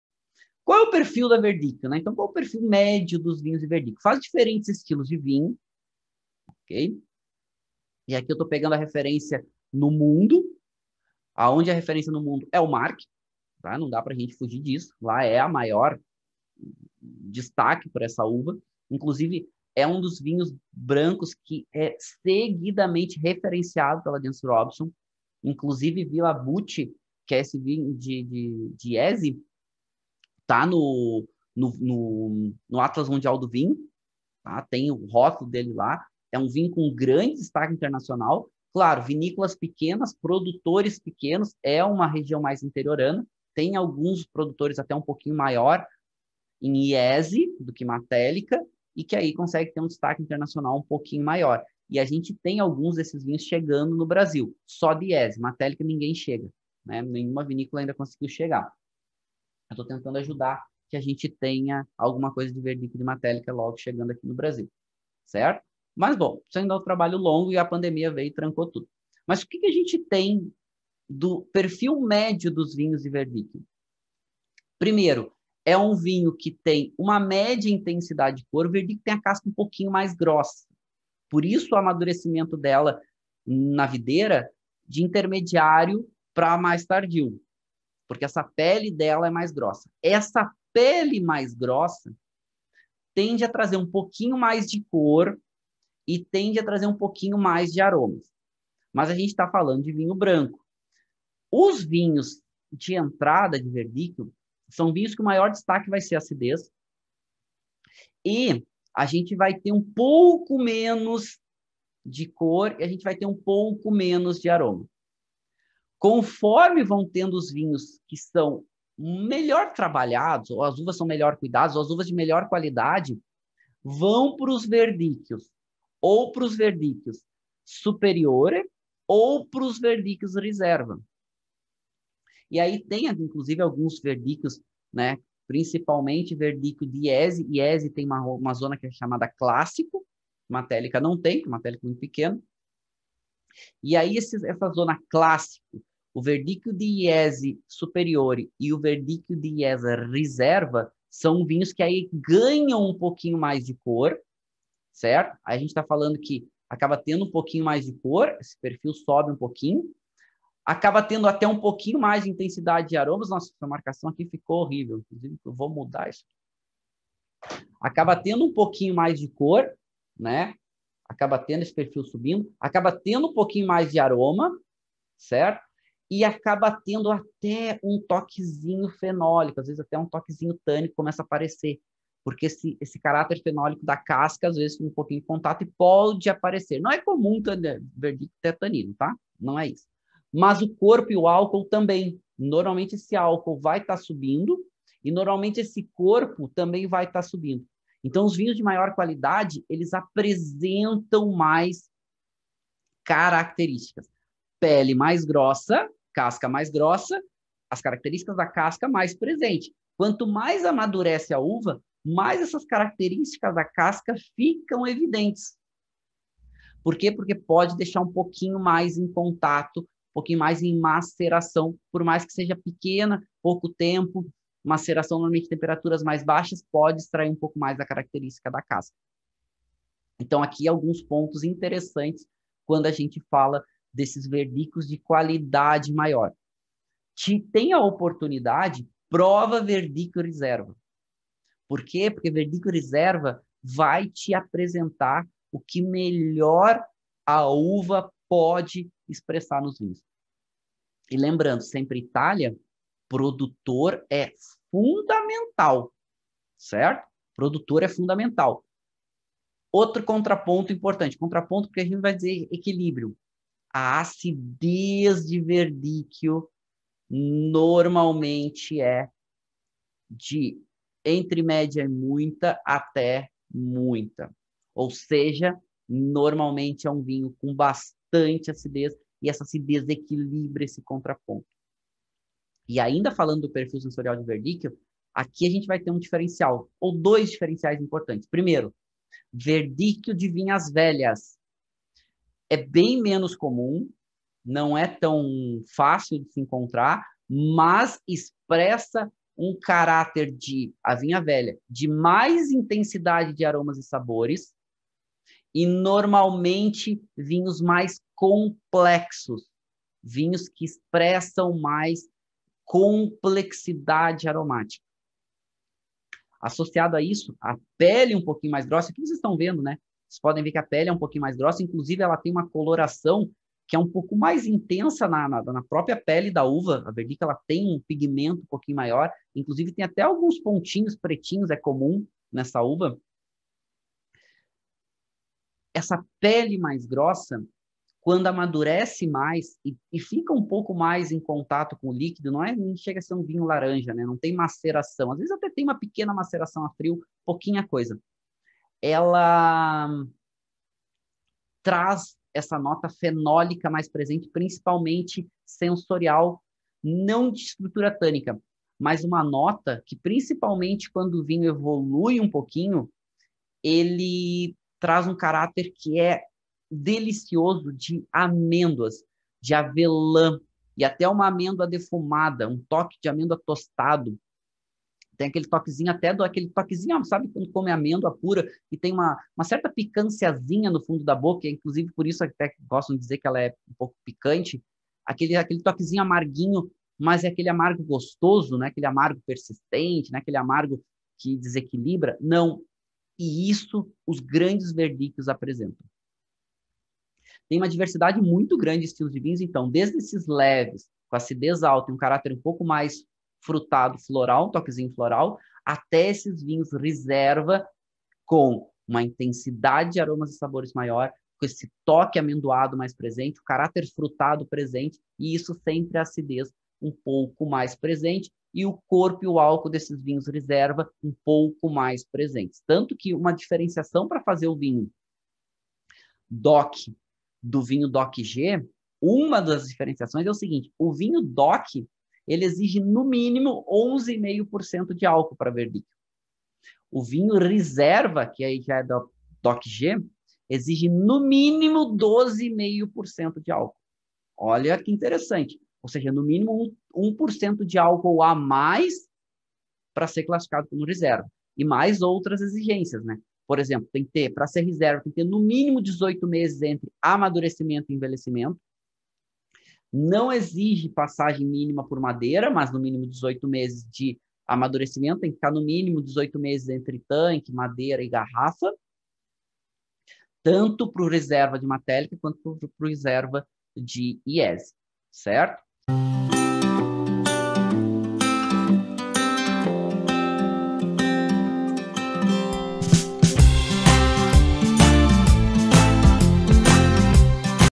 VERDICCHIO OU PEVERELLA - PERFIL SENSORIAL DOS VINHOS - PROF. MARCELO VARGAS - MINUTOS DE VINHO
Verdicchio é a uva branca mais premiada da Itália. Responsável por estilos que vão de leves com toques frutados e florais, até os mais valorizados pela concentração e intensidade. Também é conhecida pelo potencial de guarda dos produtos com classificação Riserva.

No Brasil, vem ganhando destaque nos Caminhos de Pedra, na Serra Gaúcha, onde é conhecida pelo sinônimo, Peverella. A variedade nesse local é tão relevante, que é citada no livro “Wine Grapes”, de Jancis Robinson.

Nesta aula sobre vinhos , o professor Marcelo Vargas explica sobre a variedade e o perfil dos seus vinhos..